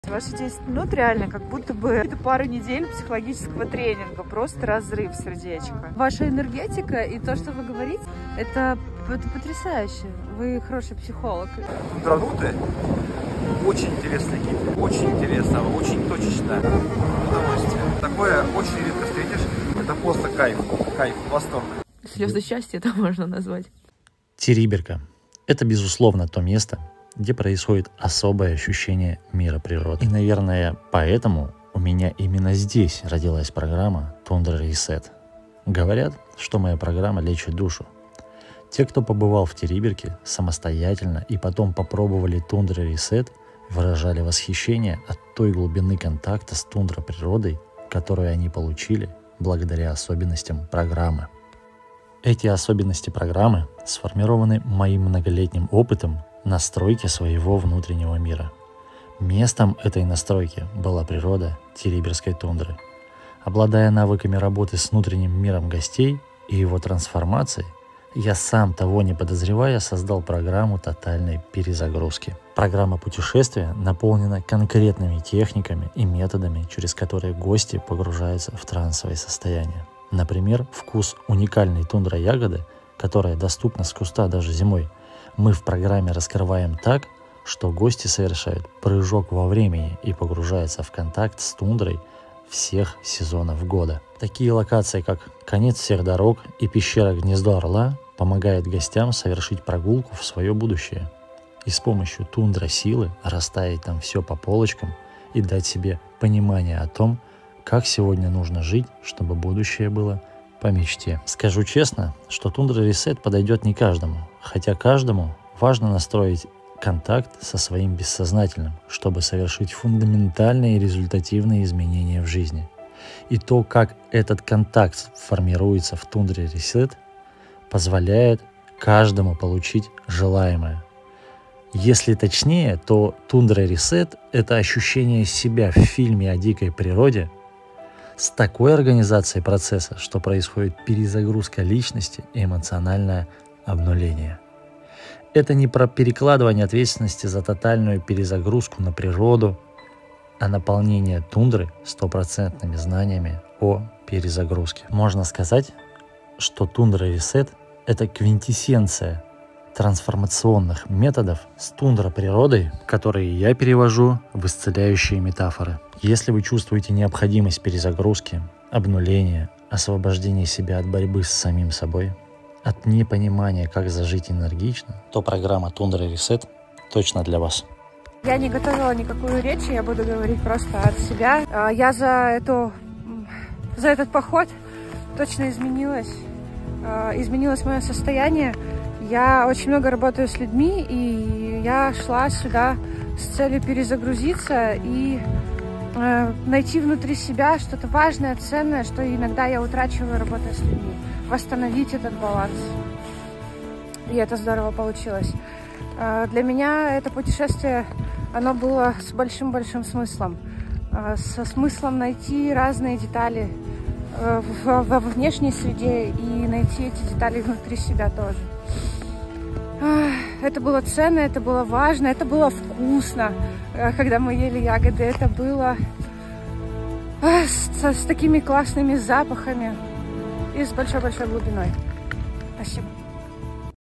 Перезагрузочные путешествия в Териберку.
Териберка. Край земли. Место силы🌎💪

Тундра-RESET - это:✨ Путешествие в Териберку с местным "шаманом"✨ Мягкая перезагрузка через исцеляющие метафоры связанные с природой 🔄🌳✨Вхождение в состояние покоя ума и тела✨ Ощущение сияния души и тела✨ Впечатления и восторг от ощущения себя как в фильме о дикой Арктике✨ Контент о котором мечтают многие📸

Я умею превращать путешествие в значимое событие для вашей души🙏Забудь про эзотерику и ожидание чуда. Тундра-RESET – это 100% адекватный подход к перезагрузке через метафоры природы. 💯 Пишите «+» и вы уже в Териберке.